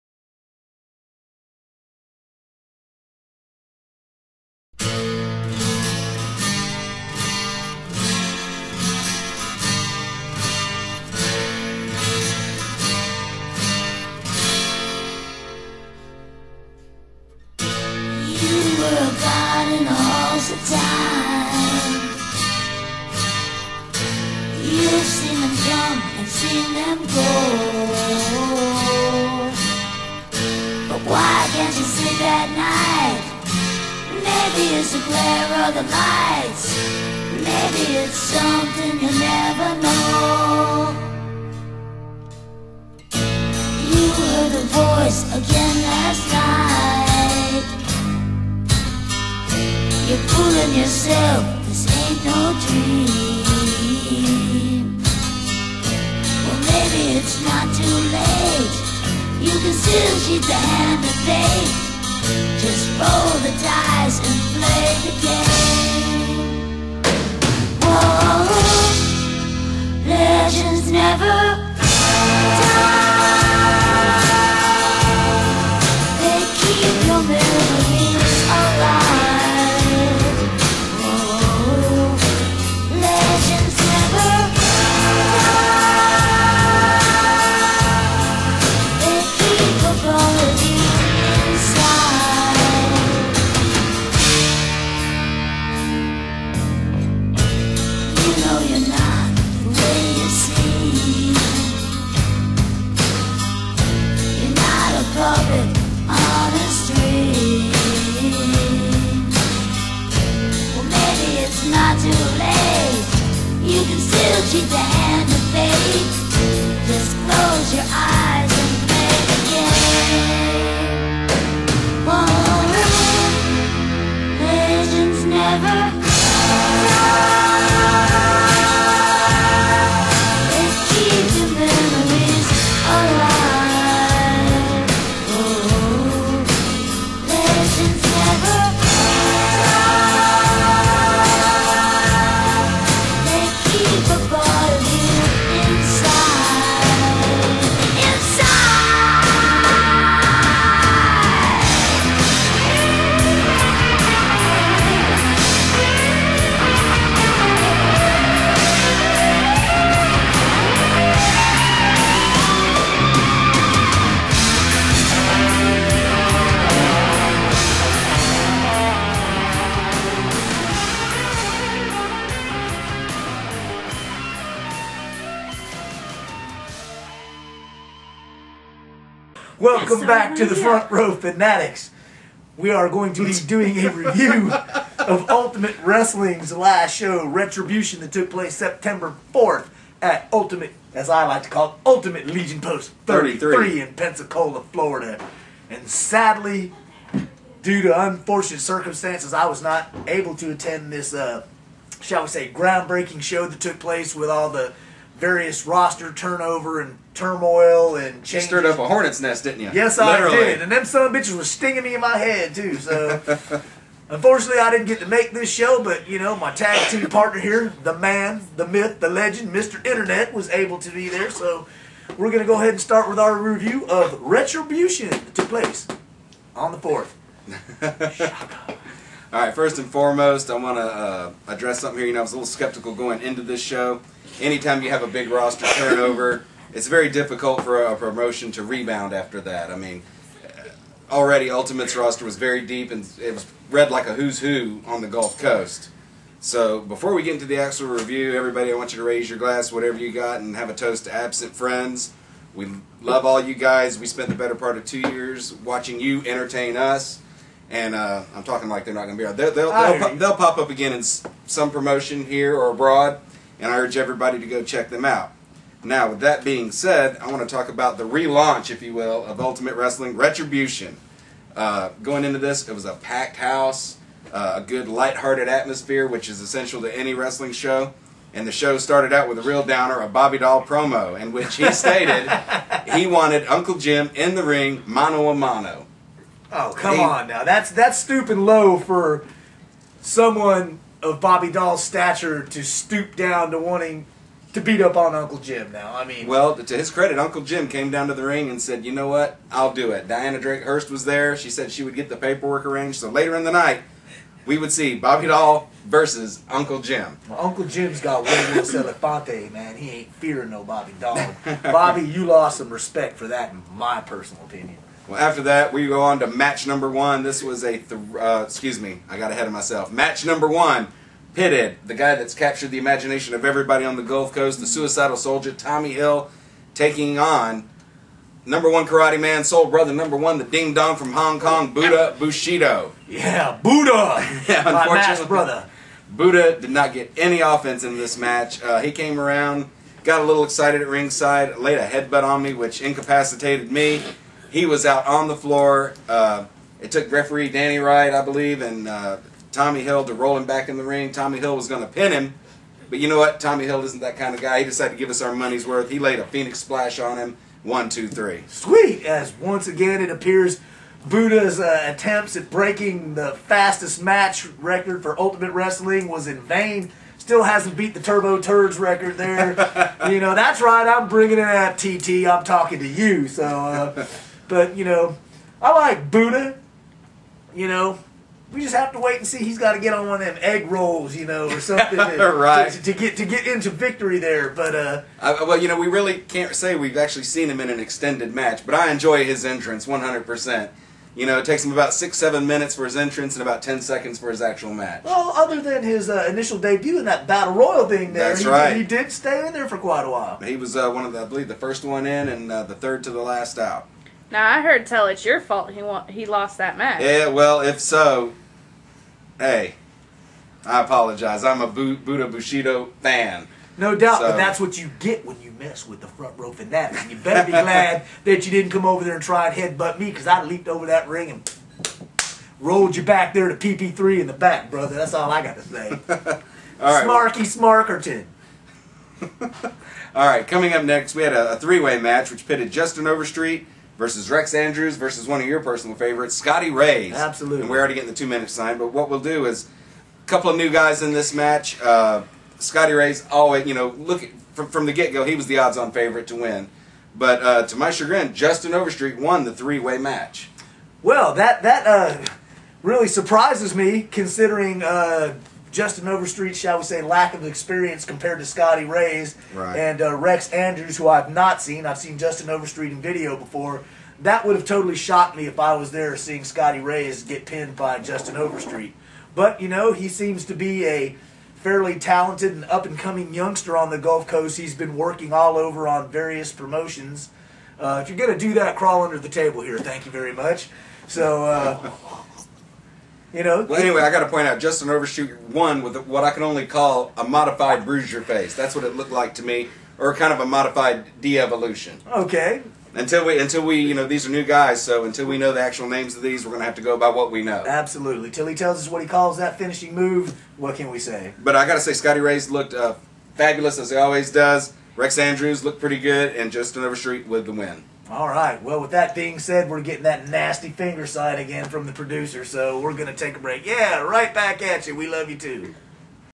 Front Row Fanatics Review of Ultimate wrestling "Retribution"
Mr.Internet and The Gargoyle talk about retribution that took place on saturday september 4th